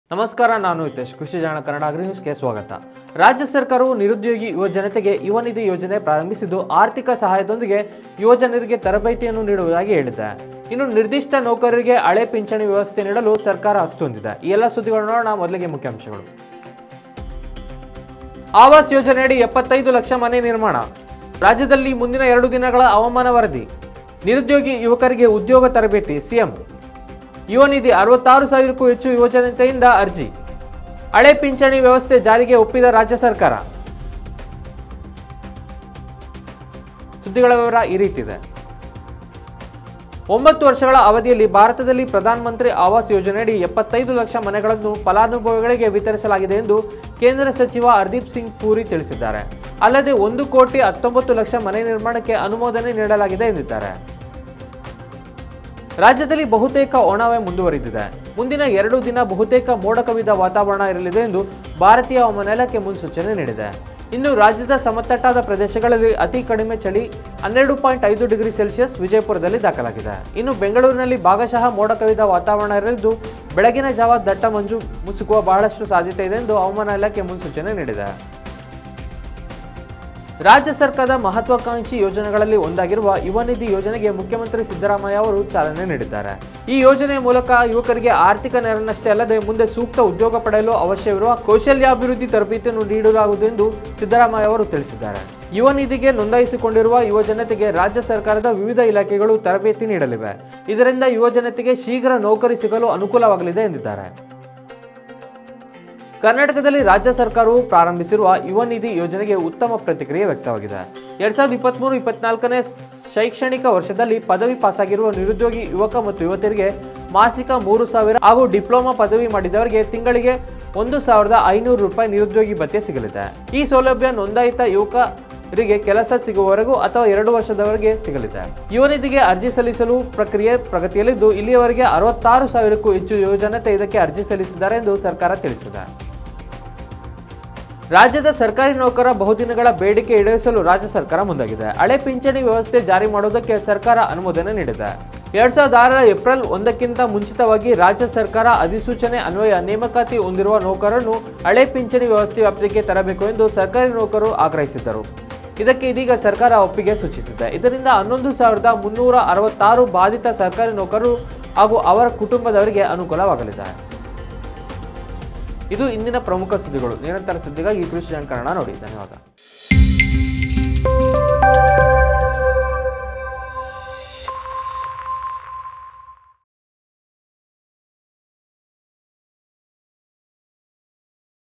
Old Pension Scheme | ಹಳೇ ಪಿಂಚಣಿ ಜಾರಿಗೆ ಒಪ್ಪಿದ ಸರ್ಕಾರ | ನಿರುದ್ಯೋಗಿಗಳಿಗೆ Good News | Housing Scheme
ಕೃಷಿ ಜಾಗರಣ ಕನ್ನಡ ಅಗ್ರಿನ್ಯೂಸ್‌ಗೆ ಸ್ವಾಗತ. ರಾಜ್ಯ ಸರ್ಕಾರವು ನಿರುದ್ಯೋಗಿ ಯುವಜನತೆಗೆ ಯುವನಿಧಿ ಯೋಜನೆ ಪ್ರಾರಂಭಿಸಿದ್ದು, ಆರ್ಥಿಕ ಸಹಾಯದೊಂದಿಗೆ ಯುವಕರಿಗೆ ತರಬೇತಿಯನ್ನೂ ನೀಡುವುದಾಗಿ ಹೇಳಿದೆ. ಇನ್ನು ನಿರ್ದಿಷ್ಟ ನೌಕರರಿಗೆ ಹಳೇ ಪಿಂಚಣಿ ವ್ಯವಸ್ಥೆ ನೀಡಲು ಸರ್ಕಾರ ಅಸ್ತು ಎಂದಿದೆ ಈ ಎಲ್ಲ ಸುದ್ದಿಗಳನ್ನು ನೋಡಿ

1. ಆವಾಸ್ ಯೋಜನೆಯಡಿ 75 ಲಕ್ಷ ಮನೆ ನಿರ್ಮಾಣ
2. ರಾಜ್ಯದಲ್ಲಿ ಮುಂದಿನ ಎರಡು ದಿನಗಳ ಹವಾಮಾನ ವರದಿ
3. ನಿರುದ್ಯೋಗಿ ಯುವಕರಿಗೆ ಉದ್ಯೋಗ ತರಬೇತಿ: ಸಿ.ಎಂ
4. ಯುವನಿಧಿ: 66 ಸಾವಿರಕ್ಕೂ ಹೆಚ್ಚು ಯವಜನತೆಯಿಂದ ಅರ್ಜಿ
5. ಹಳೇ ಪಿಂಚಣಿ ವ್ಯವಸ್ಥೆ ಜಾರಿಗೆ ಒಪ್ಪಿದ ರಾಜ್ಯ ಸರ್ಕಾರ

Welcome to Krishi Jagran Kannada Agrinews. The state government has launched a youth fund scheme for unemployed youth and said it will also provide training to the youth with financial assistance. See all these news that the government has decided to provide old pension system to certain employees
1. Construction of 75 lakh houses under Awas Yojana
2. Weather report for the next two days in the state
3. Job training for unemployed youth: C.M
4. Yuvanidi: Application from more than 66 thousand youth
5. State Government agreed to implement old pension system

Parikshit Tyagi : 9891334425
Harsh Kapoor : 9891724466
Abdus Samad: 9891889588